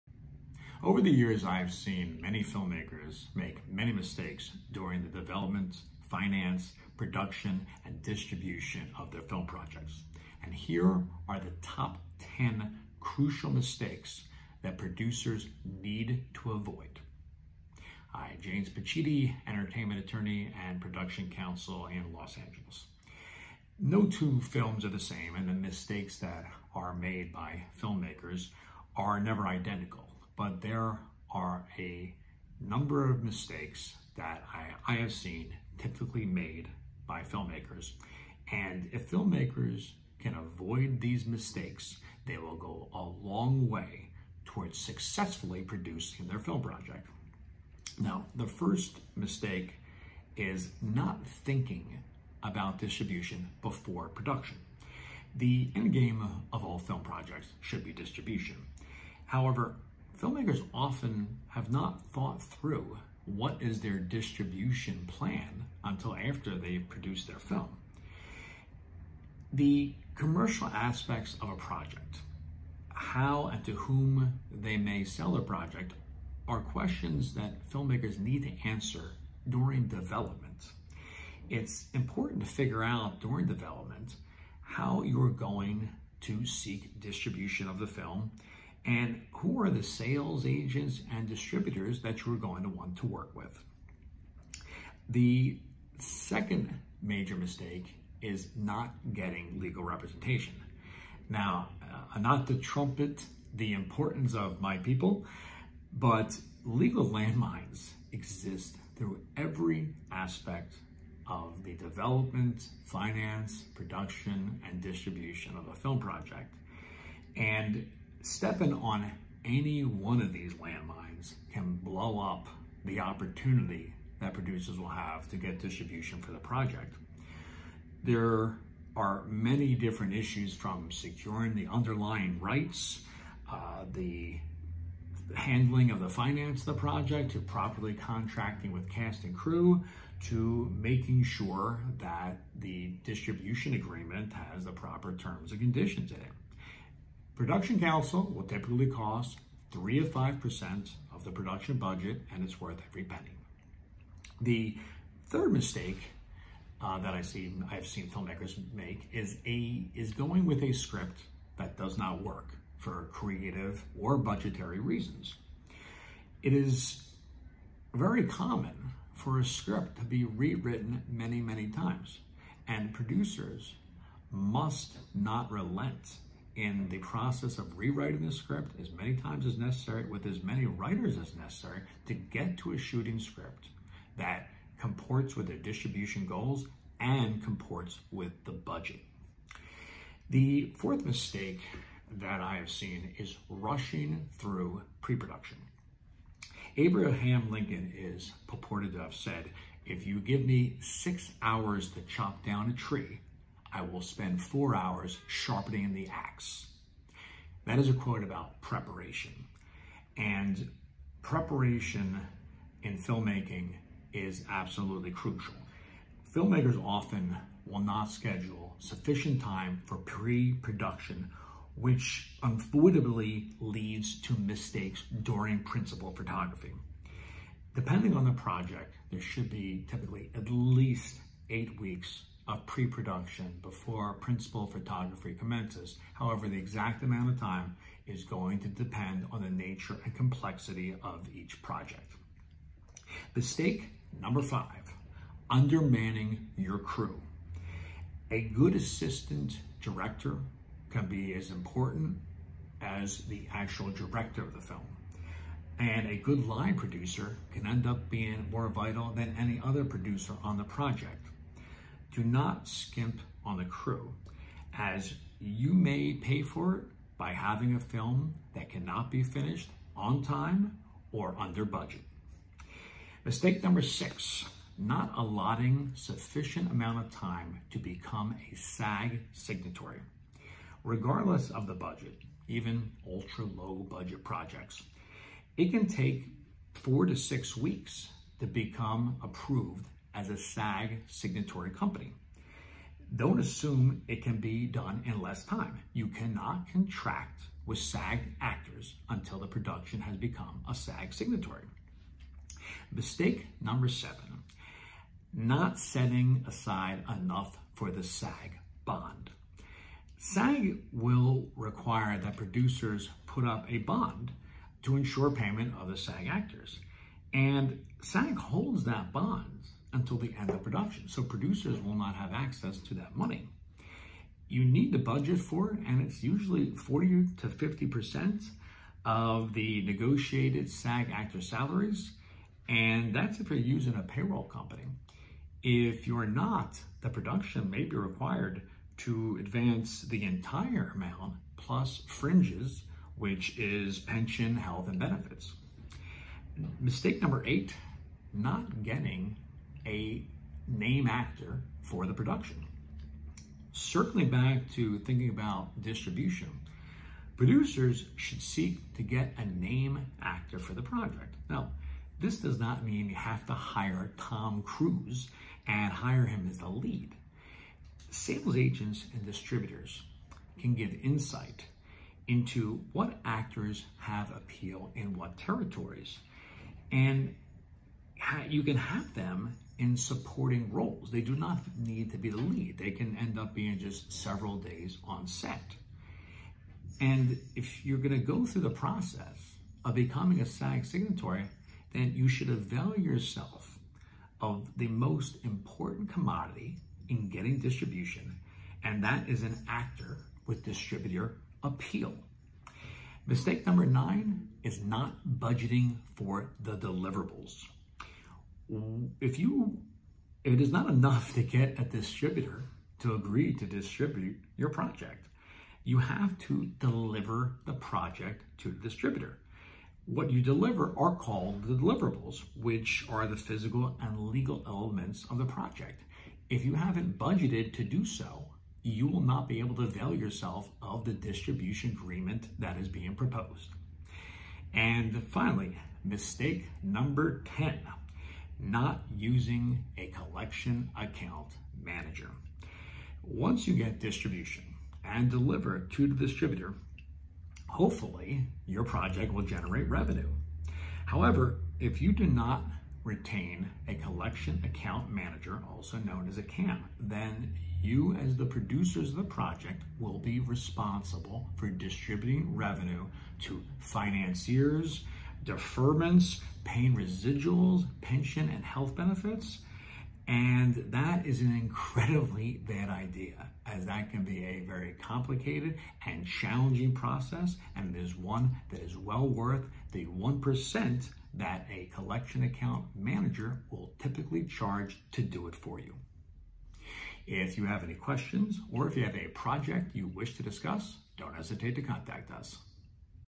TOP 10 MISTAKES FILM PRODUCERS MUST AVOID
No two films are the same & the mistakes made by filmmakers are never identical but there are a number of general mistakes that I have seen & if filmmakers avoid these mistakes, they will have a long way toward successfully producing their film.

Mistake #1: Not Thinking About Distribution – Before Production. The end game of all film projects should be distribution. However, often filmmakers have not thought about the commercial aspects of their project & how & to whom they may seek to sell their project. It’s important to figure out during development how you are going to seek distribution for your film & who are the sales agents & distributors you will look to work with.

Mistake #2: Not Getting Legal Representation. Not to trumpet the importance of my people too much, but legal landmines exist throughout every aspect of the development, finance, production, & distribution of a film & stepping on any one of these landmines can blow up your chances of ever selling your film from not properly securing the underlying rights to a script or a book, to mishandling the financing of the project, to not properly contracting with cast & crew. Production counsel will typically cost from 3-5% of the budget & it’s worth every penny.

Mistake #3: A Script That Does Not Work. It is very common for a script to be rewritten several times to get to the shooting script & often with several different writers. The script also must be cleared by an attorney for legal issues & it must comport with the budget for the project.
Mistake #4: Rushing Through Pre-Production. Abraham Lincoln is purported to have said “Give me six hours to chop down a tree & I will spend the first four sharpening the ax.” It’s a quote about preparation & often filmmakers will not schedule sufficient time for pre-production which will unavoidably lead to mistakes. Depending on the project, there typically should be at least 8 weeks of pre-production though that will vary depending on the scope & complexity of the project.

Mistake #5: Under-Manning Your Crew. A good assistant director can be as important as the director just as a good line producer can be more vital than any producer. Do not skimp on the crew as you may pay for it by having a film that cannot be finished on time & within budget.
Mistake #6: Not Sufficient Time To Become A SAG Signatory. Regardless of the budget, even ultra-low-budget or micro-budget films, it can take 4-6 weeks to become a SAG signatory from the date of the initial application. Don’t assume it can be done in less time. You cannot contract with SAG actors until you become a SAG signatory.

Mistake #7: Not Setting Aside Funds For The SAG Bond. SAG will require that producer put up a bond to ensure payment of the SAG actors & SAG holds that bond until the end of production. You need to budget for it & it is usually 40-50% of negotiated SAG actor salaries if you are using a payroll company. If you are not, the production may be required to advance the entire amount plus fringes, which pension, health & benefits.

Mistake #8: Not Getting A Name Actor For SAG Productions. Circling back to thinking about distribution, producers should seek to get a “name” actor for their project. This does not mean you have to hire Tom Cruise & hire him as the lead. Sales agents & distributors can give insight into what actors have appeal in what territories & you can have them in a supporting role. If you are going through the process of becoming a SAG signatory producer, you should avail yourself of the most important commodity in getting distribution, and that is an actor with distributor appeal.

Mistake #9: Not Budgeting for the Deliverables. It is not enough to get a distributor to agree to distribute your project. You have to deliver the project to the distributor. What you deliver are called the deliverables – the physical & legal elements of the project.

Mistake #10: Not using a Collection Account Manager: Once you get distribution & deliver the film, hopefully, it will generate revenue. However, if you do not retain a collection account manager also known as a CAM, you will be responsible for distributing the revenue to financiers, profit participants & deferments, & paying residuals & pension & health benefits for union cast & crew. That is an incredibly bad idea & it is worth the 1% typically charged to have a third party handle it.